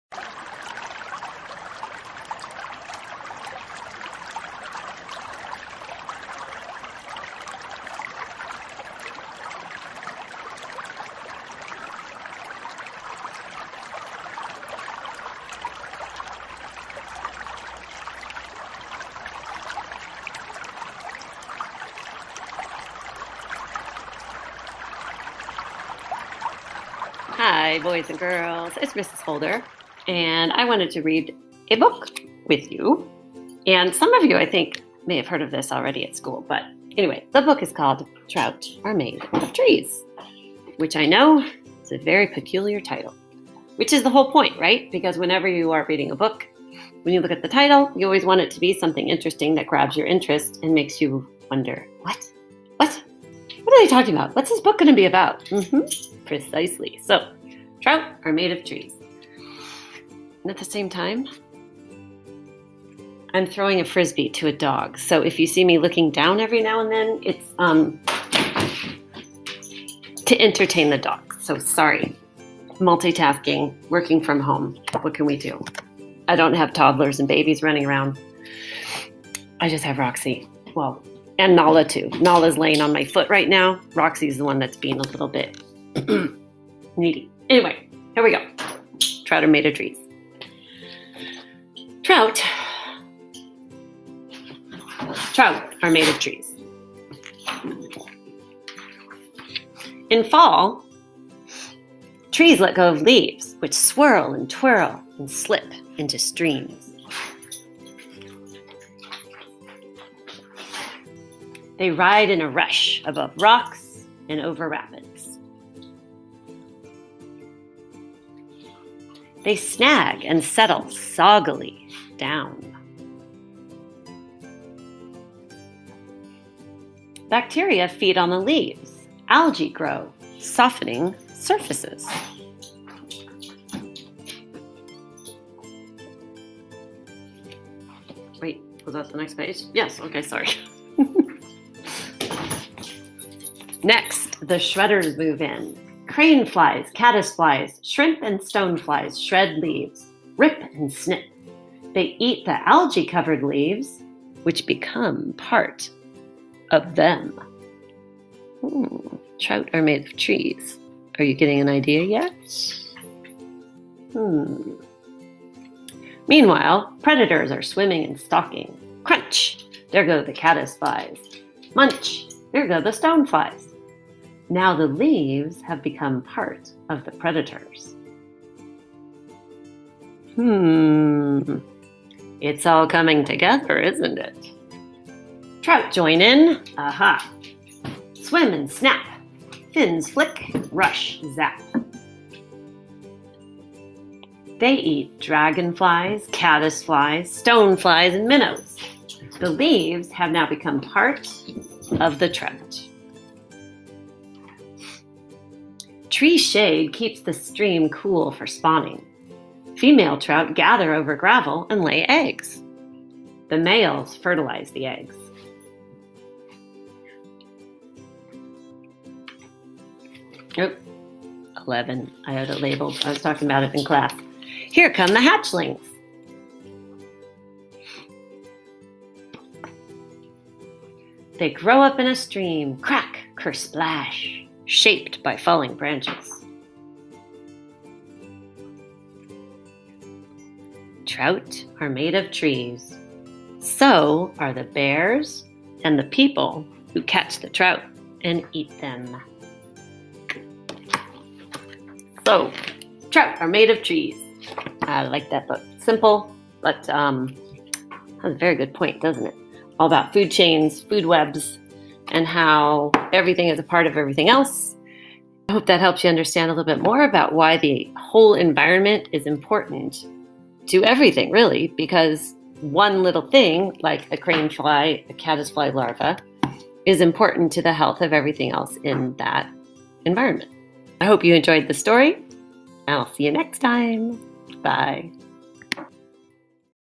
Trout are Made of Trees
by April Pulley Sayre
Illustrated by Kate Endle
Read by Mrs. Holder, PUSD Science Specialist

I use this story in the science lab as part of our Trout in the Classroom Project.  This story is about the interconnectedness of plants and animals via the food web.